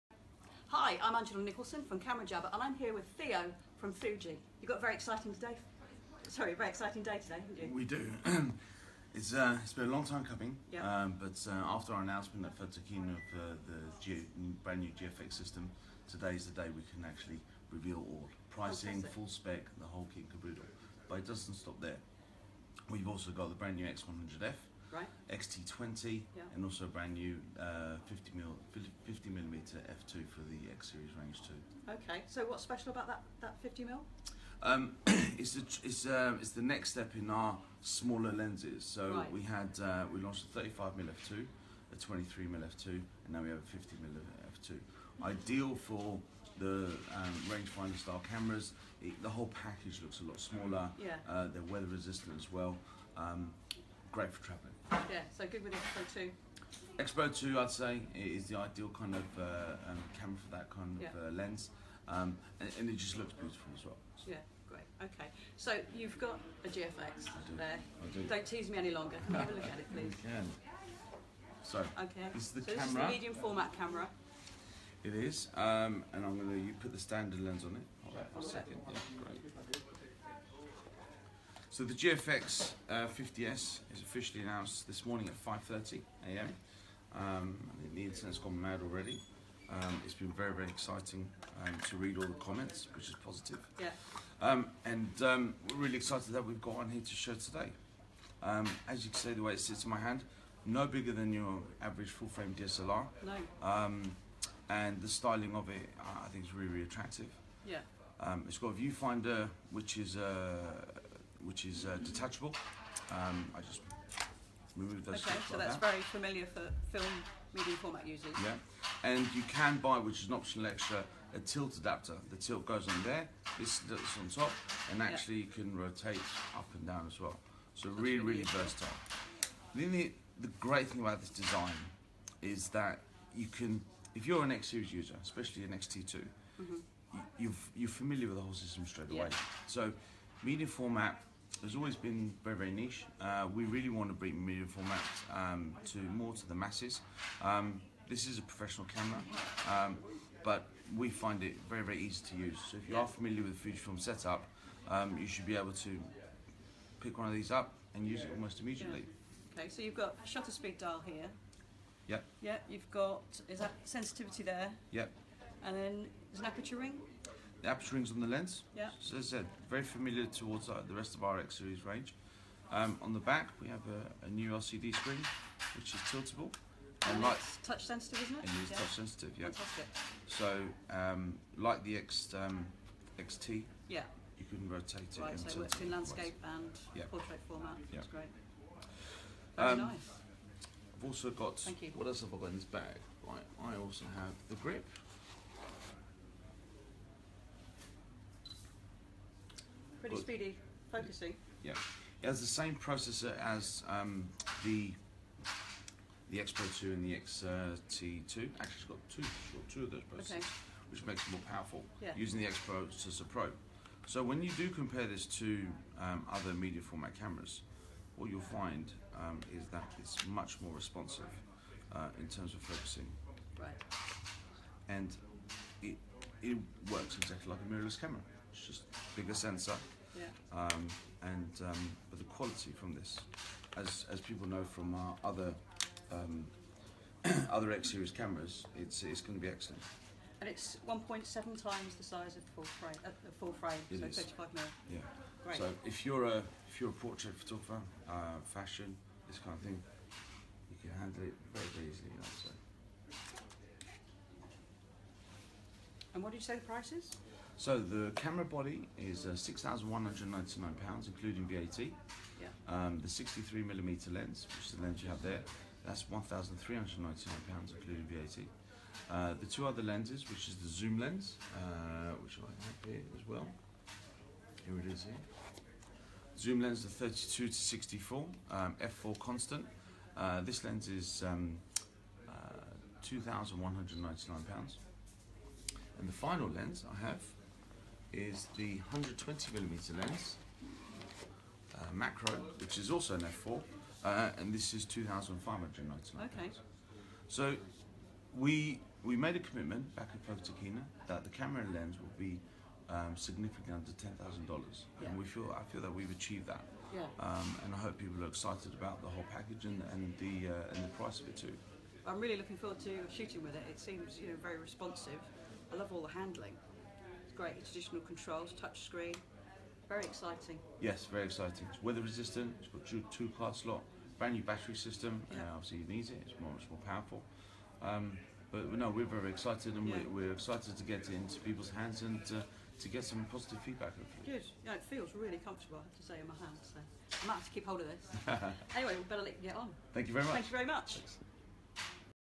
Fujifilm explains the GFX 50S key features
Theo Georghiades from Fujifilm UK walks us through some of the key features of the new mirrorless medium format Fuji GFX 50S and its GF lenses during a Facebook Live video. In the video  it's said that the camera had a dual processor which is incorrect. The early plans for GFX was to have a dual processor but it's been confirmed that it actually has a single processor.